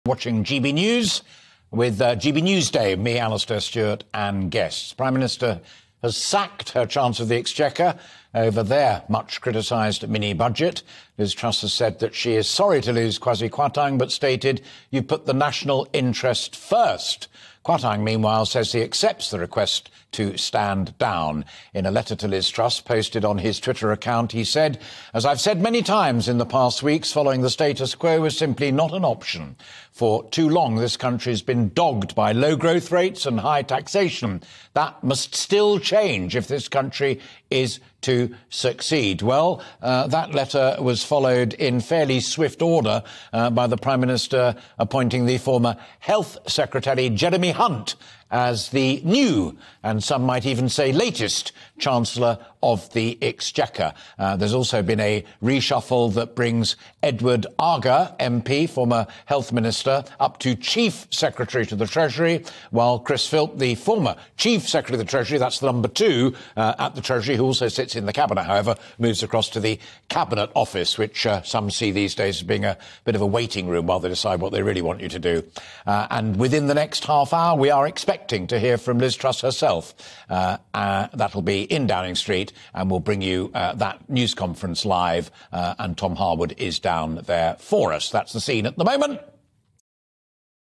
Former Health Secretary Jeremy Hunt will replace Kwasi Kwarteng as Chancellor of the Exchequer
Prime Minister Liz Truss has announced that Former Health Secretary Jeremy Hunt will replace Kwasi Kwarteng as Chancellor of the Exchequer.